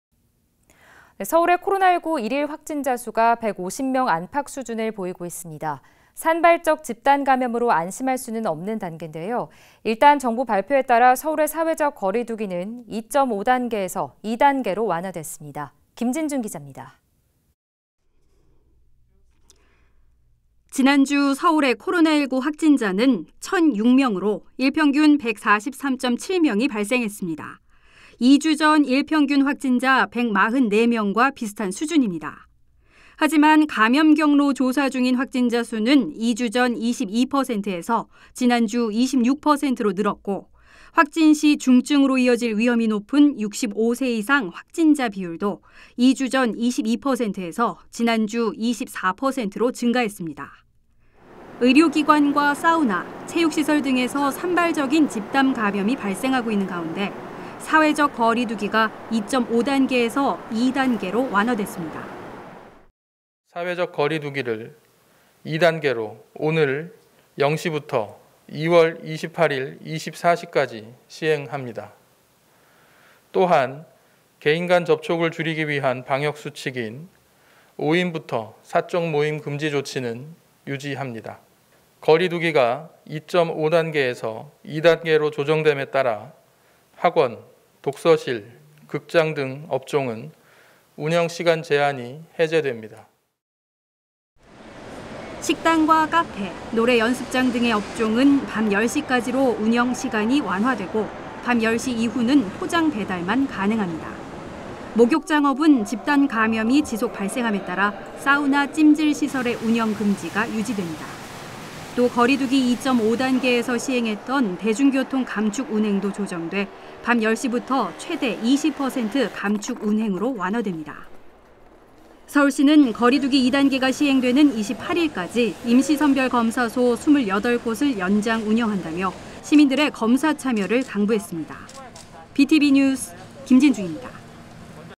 재확산 우려 속 '거리두기 2단계' 완화_SK broadband 서울뉴스
[B tv 서울뉴스 김진중기자]

[앵커멘트]
서울의 코로나19 일일 확진자 수가
150명 안팎 수준을 보이고 있습니다.
산발적 집단감염으로
안심할 수는 없는 단계인데요.
일단 정부 발표에 따라 서울의 사회적 거리두기는
2.5단계에서 2단계로 완화됐습니다.
김진중 기자가 보도합니다.

[기사내용]
지난주 서울의 코로나19 확진자는 1천 6명으로
일평균 143.7명이 발생했습니다.

2주 전 일평균 확진자 144명과
비슷한 수준입니다.

하지만 감염경로 조사 중인 확진자 수는
2주 전 22.1%에서 지난주 26%로 늘었고,
확진 시 중증으로 이어질 위험이 높은
65세 이상 확진자 비율도 2주전 21.7%에서
지난주 23.9%로 증가했습니다.

의료기관과 사우나, 체육시설 등에서
산발적인 집단감염이 발생하고 있는 가운데,
사회적 거리두기가 2.5단계에서 2단계로
완화됐습니다.

송은철/서울시 감염병관리과장 (2월 15일 발표)
(사회적 거리두기를 2단계로
오늘 0시부터 2월 28일 24시까지 시행합니다.
또한, 개인 간 접촉을 줄이기 위한 방역수칙인
'5인부터 사적모임 금지' 조치는 유지합니다.
거리두기가 2.5단계에서 2단계로 조정됨에 따라
학원, 독서실, 극장 등 업종은
운영시간 제한이 해제됩니다.)

식당과 카페, 노래연습장 등의 업종은
밤 10시까지로 운영시간이 완화되고,
밤 10시 이후는 포장, 배달만 가능합니다.

목욕장업은 집단감염이 지속 발생함에 따라
사우나, 찜질 시설의 운영 금지가 유지됩니다.

또, 거리두기 2.5단계에서 시행했던
대중교통 감축 운행도 조정돼
밤10시부터 최대 20% 감축 운행으로 완화됩니다.

서울시는 거리두기 2단계가 시행되는 28일까지
임시선별검사소 28곳을 연장 운영한다며,
시민들의 검사 참여를 당부했습니다.

B tv 뉴스 김진중입니다.

(촬영/편집- 신승재 기자)